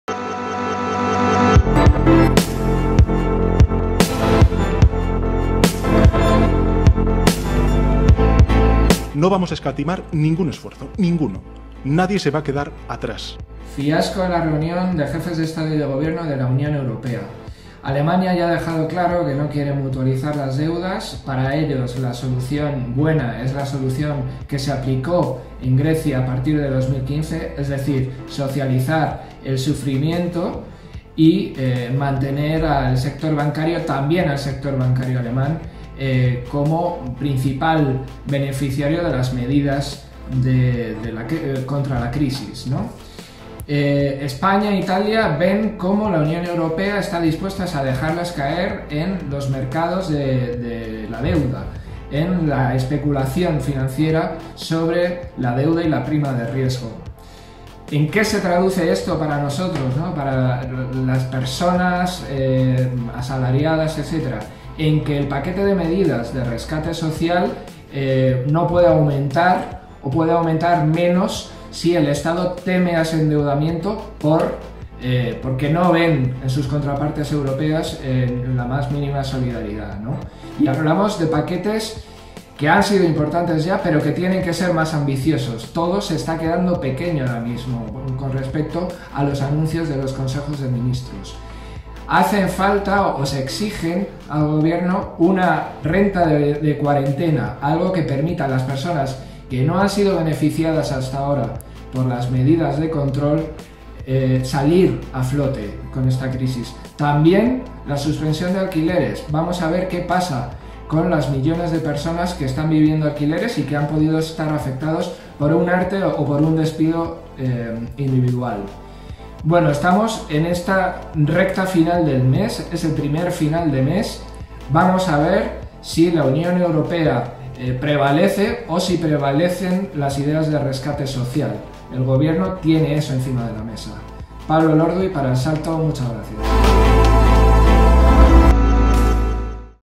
Ortodoxia liberal o rescate social. Análisis de la crisis del coronavirus
Pablo Elorduy analiza el fiasco en la reunión de jefes de Estado y de gobierno de la UE.

Alemania no quiere mutualizar las deudas y parece que la única solución es la que se le aplicó a Grecia en 2015.